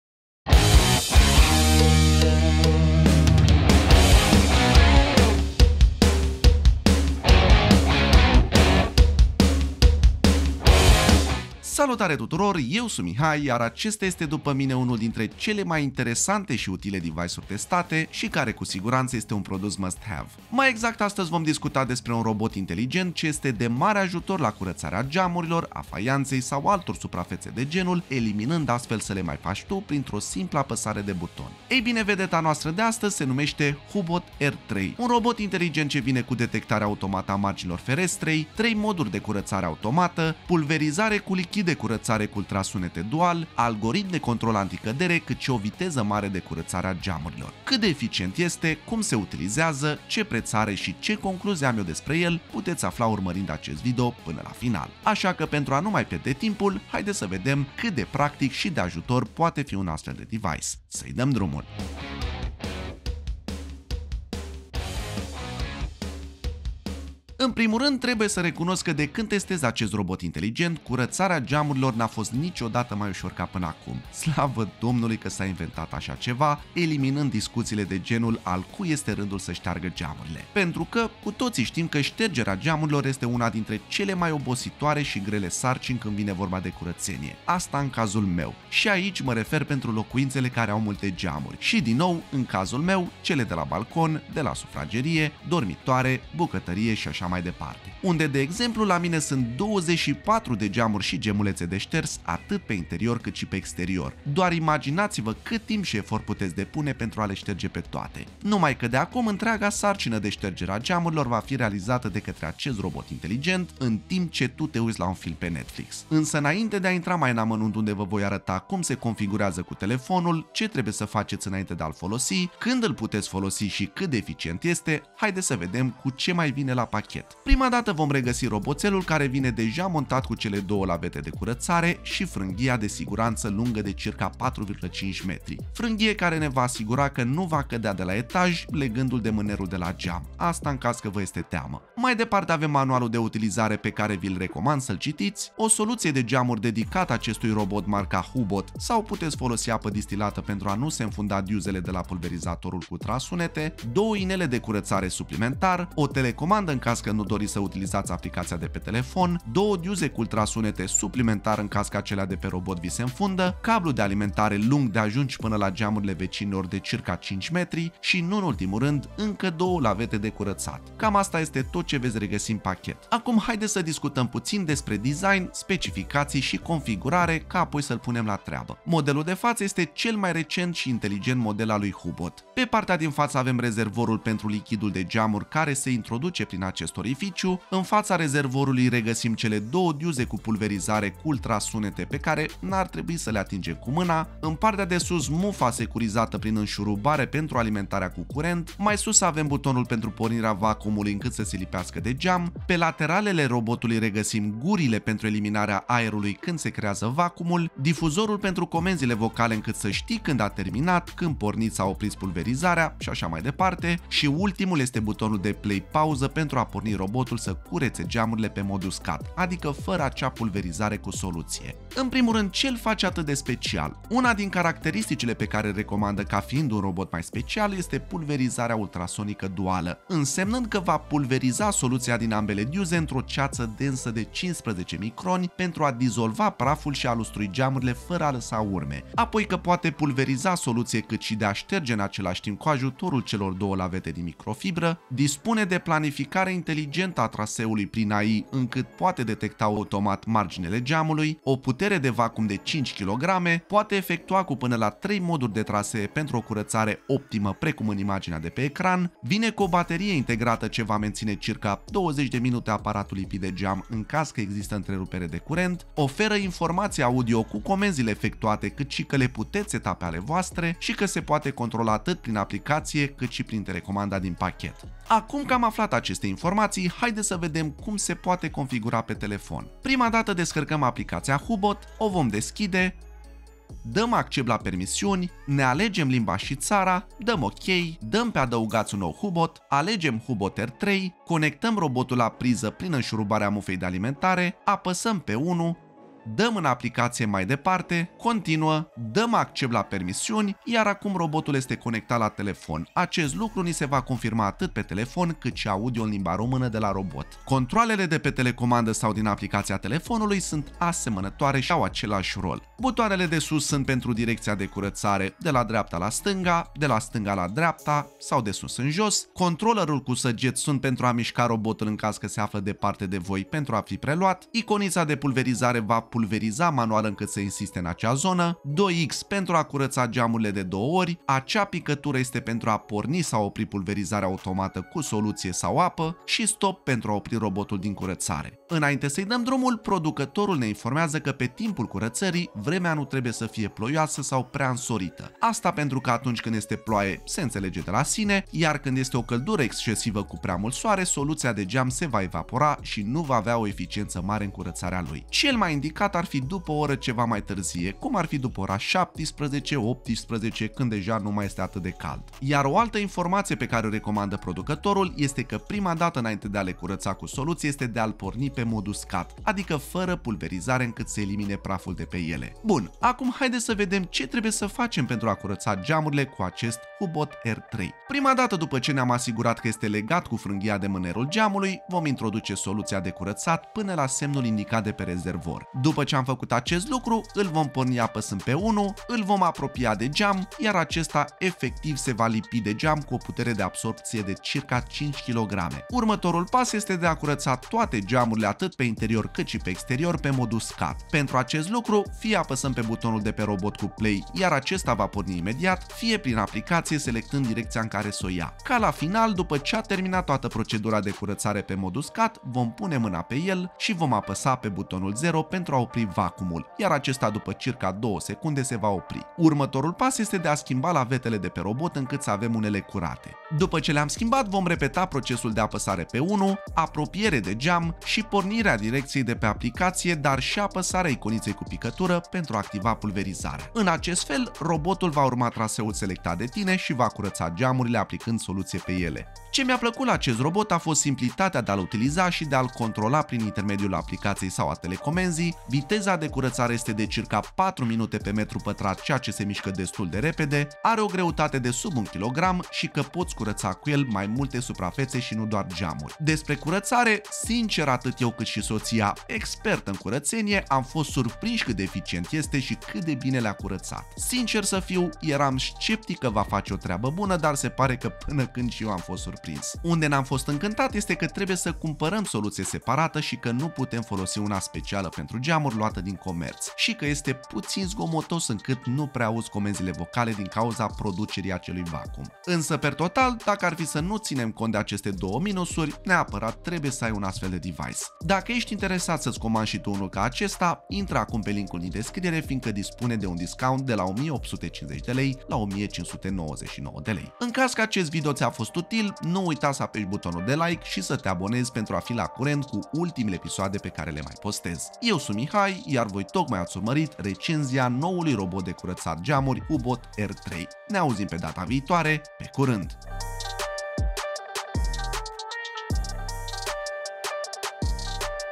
Cel mai bun ajutor la casa omului - Robot de curățat geamuri Hobot R3 [Review Română]
Astăzi vom discuta despre un robot inteligent care poate fi de mare ajutor în curățarea geamurilor, faianței sau a altor suprafețe similare.

Hobot R3 este un robot inteligent care vine cu o serie de funcții avansate. În primul rând, dispune de o detectare automată a marginilor ferestrei, ceea ce înseamnă că poate detecta marginile și poate curăța fereastra într-un mod precis și eficient.
De asemenea, are 3 moduri de curățare automată, pulverizare cu lichid de curățare cu ultrasunete dual și un algoritm de control anti-cadere.

*Acest material este sponsorizat, însă evaluarea noastră a produsului rămâne independentă și imparțială.